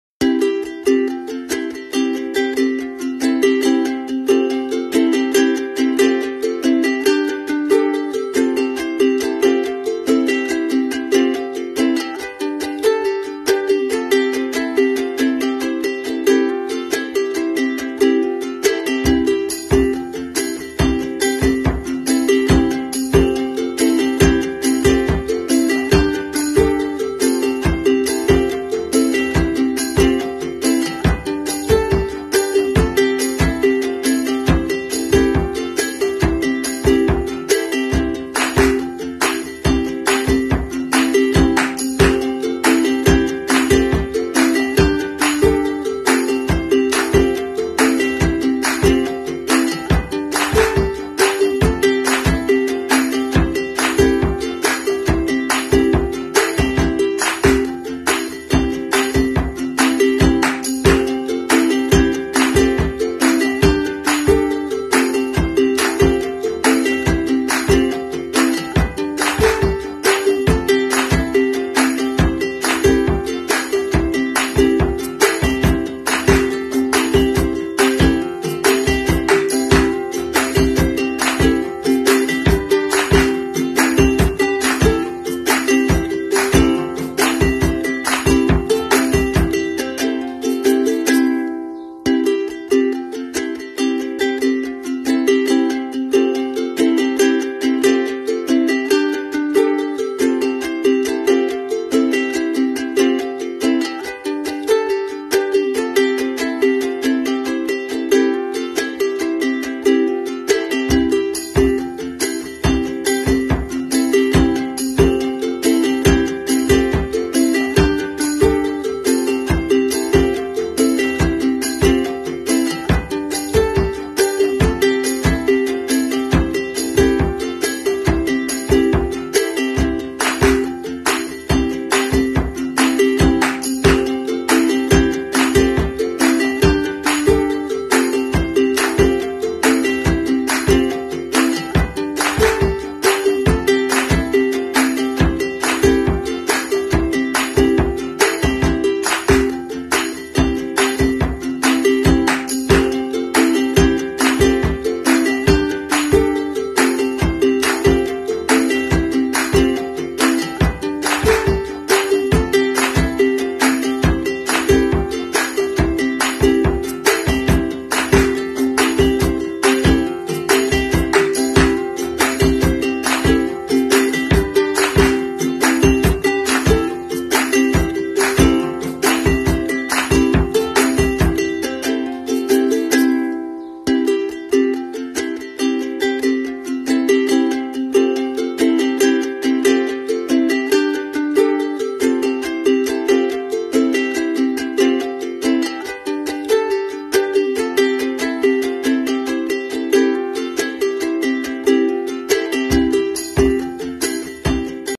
Easy Drawing peacock feather | Oil Pastel Drawing
Learn how to draw a peacock feather using oil pastel